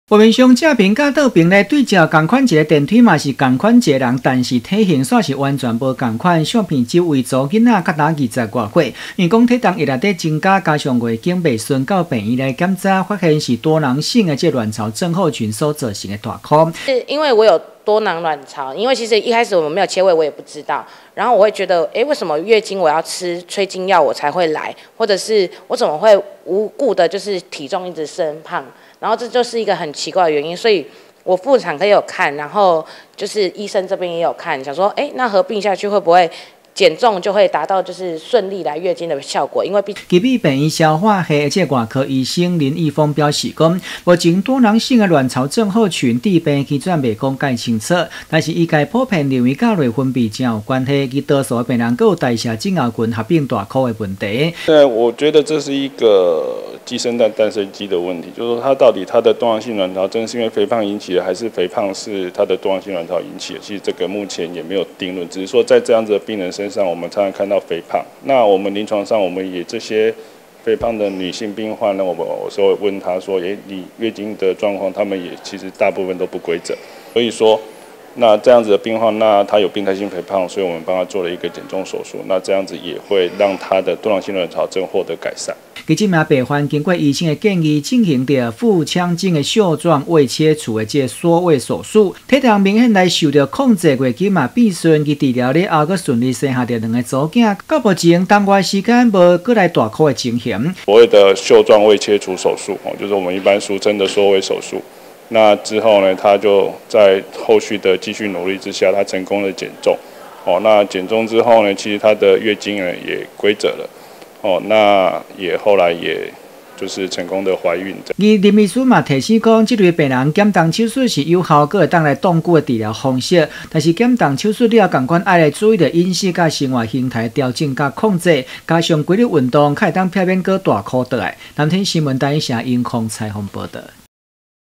多囊卵巢症候釀病態肥胖 女子縮胃手術成功減重20240112
多囊卵巢症候釀病態肥胖 女子縮胃手術成功減重
有一名20多歲的女子，月經一直不順，且體重持續變增加，重達114公斤，到奇美醫院就診，經果檢查是罹患「多囊性卵巢症候群」。在醫生評估建議之下，接受「腹腔鏡袖狀胃切除(縮胃手術)」。半年時間成功減重55公斤，月經也恢復正常，還自然受胎，一年半時間成功生了兩個女兒，透過自己飲食控制，產後也沒有再復胖。